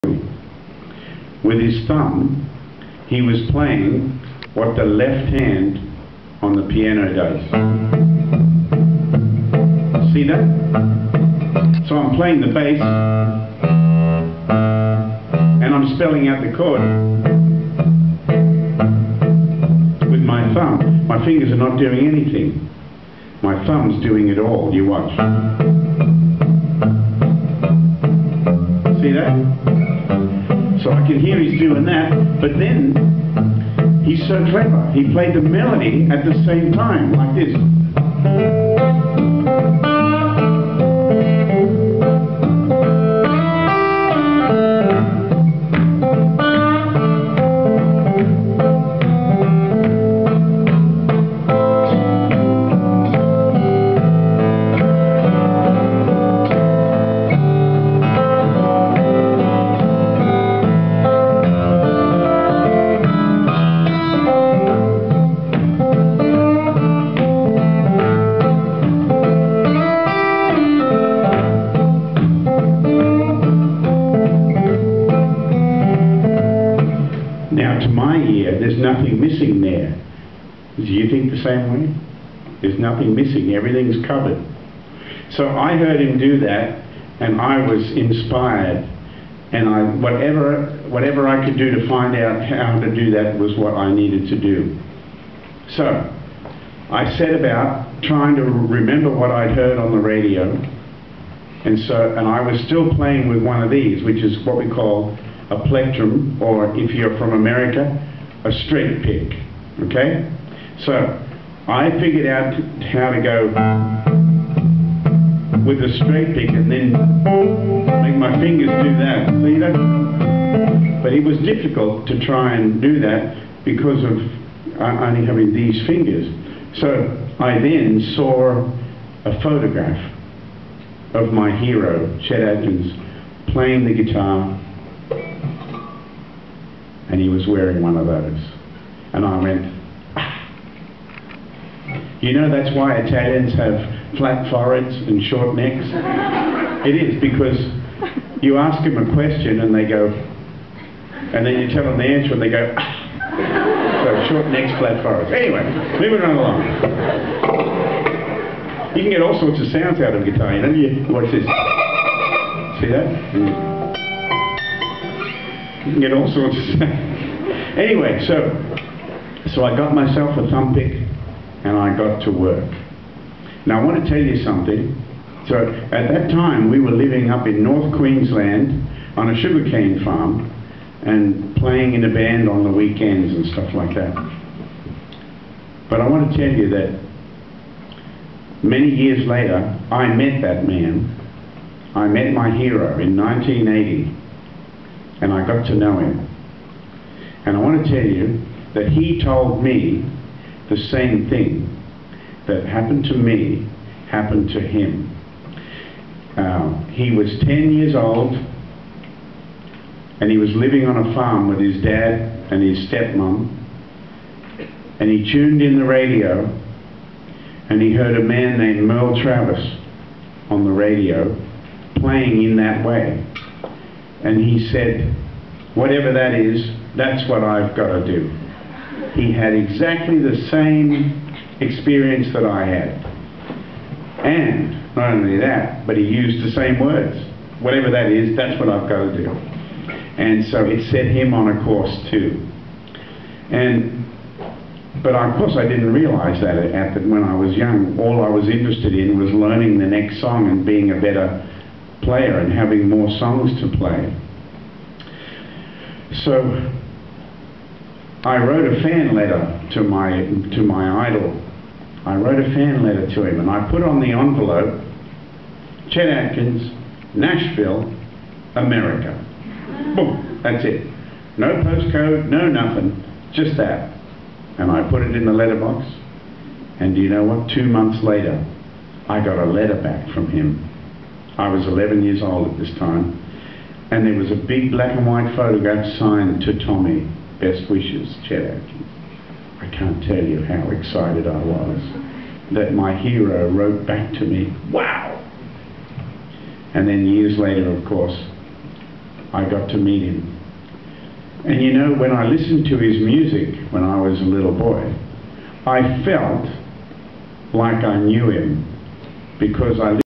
Tommy Emmanuel talks about Chet Atkins
Tommy Emmanuel visited Meriden School in Strathfield, Sydney to talk to the students and play for them. Amazing and wonderful.